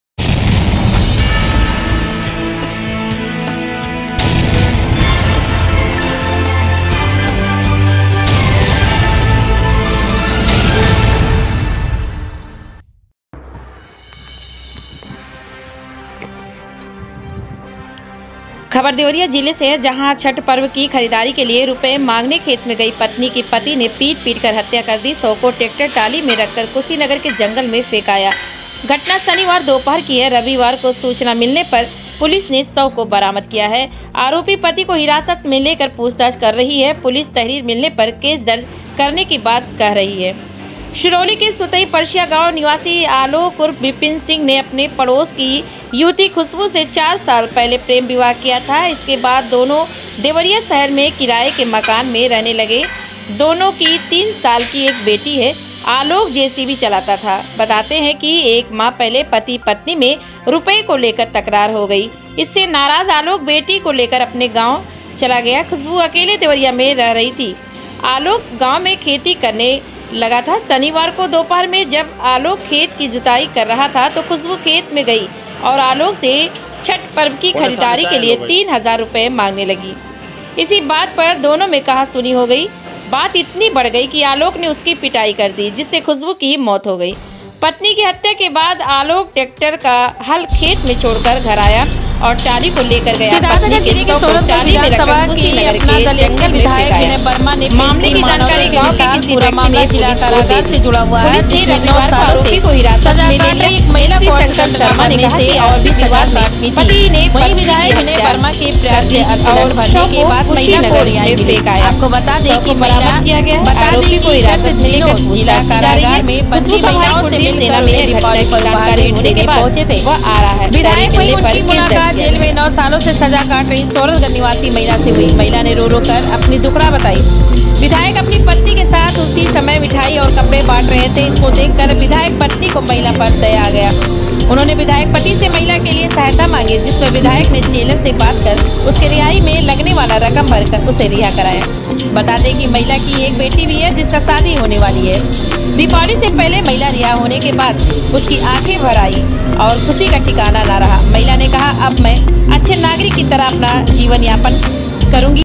ft#devriya#छठ पर्व की खरीदारी के लिए रुपये मांगने खेत में गई पत्नी की पति ने पीट-पीटकर हत्या कर दी।
uttarpradesh up ft news digital#devriya#छठ पर्व की खरीदारी के लिए रुपये मांगने खेत में गई पत्नी की पति ने पीट-पीटकर हत्या कर दी।